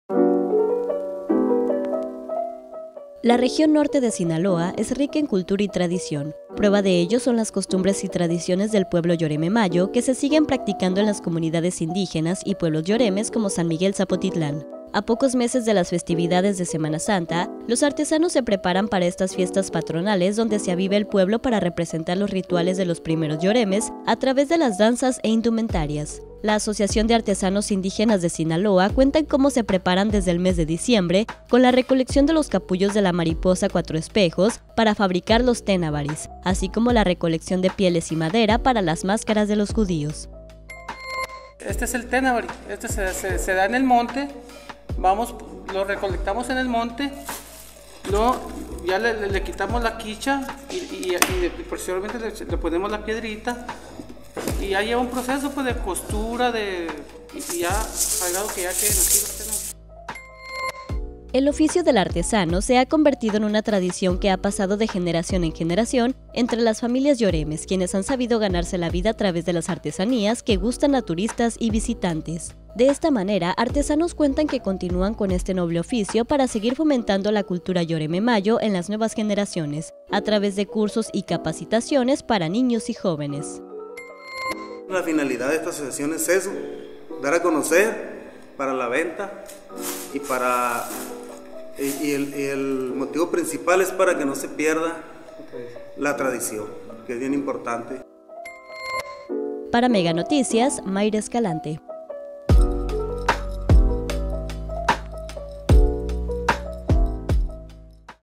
Artesanos de San Miguel se preparan para semana mayor
La región norte de Sinaloa es rica en cultura y tradición, prueba de ello son las costumbres y tradiciones del pueblo yoreme-mayo que se siguen practicando en las comunidades indígenas y pueblos yoremes como San Miguel Zapotitlán.